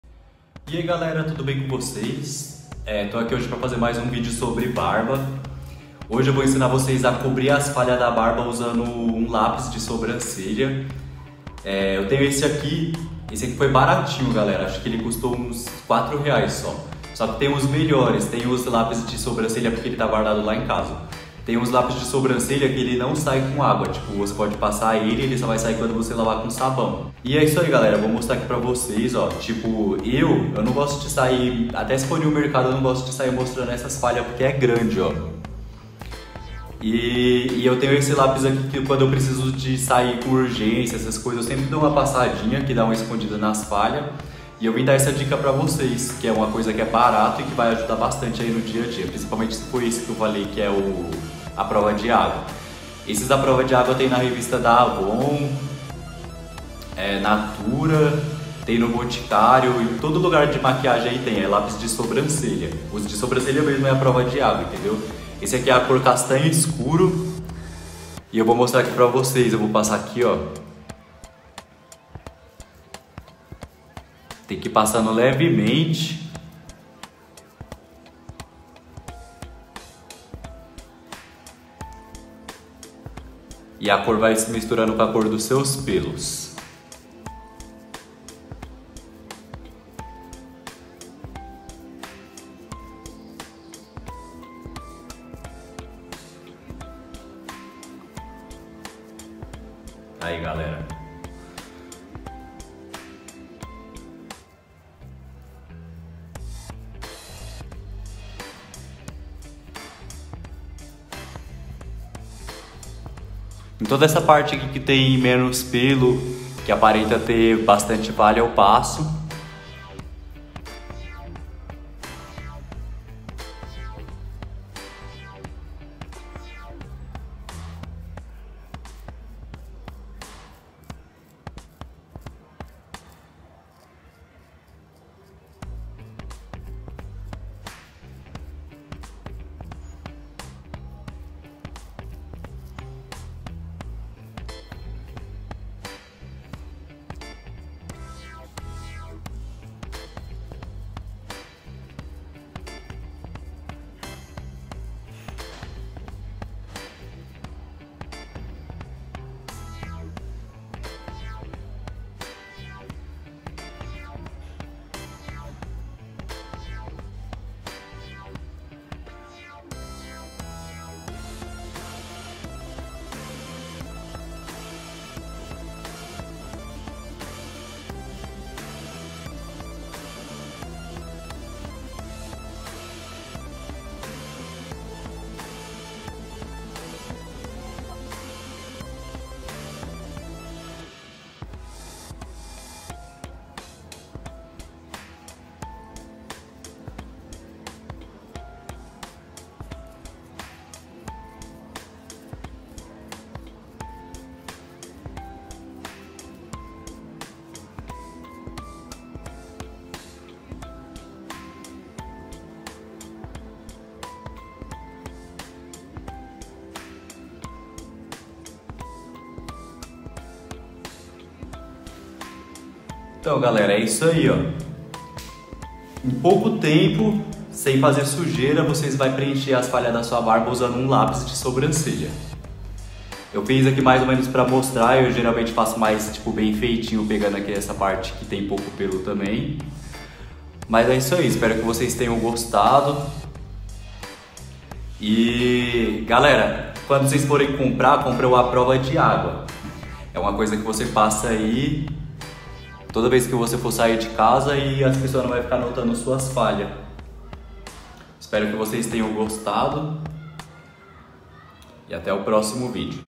COMO COBRIR FALHAS NA BARBA USANDO UM LÁPIS DE SOBRANCELHA
Eae galera,
Fiz esse video para mostrar uma maneira de cobrir as falhas na barba usando um lápis de sobrancelha. O resultado fica incrível 😀
Dúvidas pode deixar nos comentários ou chamar no insta @sobrinho.leonardo